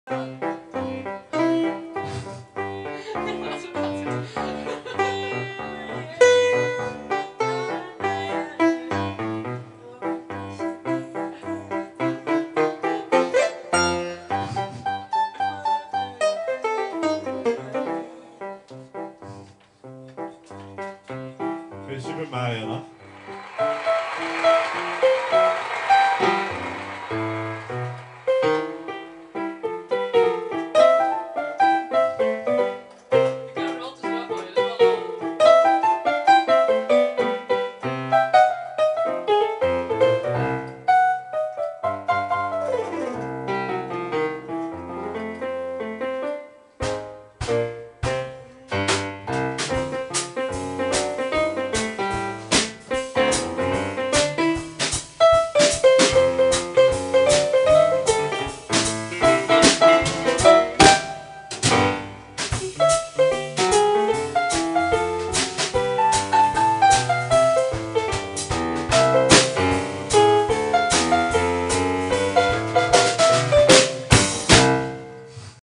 Neon Mind pianist Marijn Nijenhuis
Neon Mind pianist Marijn plays during the repetition on 08/02/2009. Enjoy!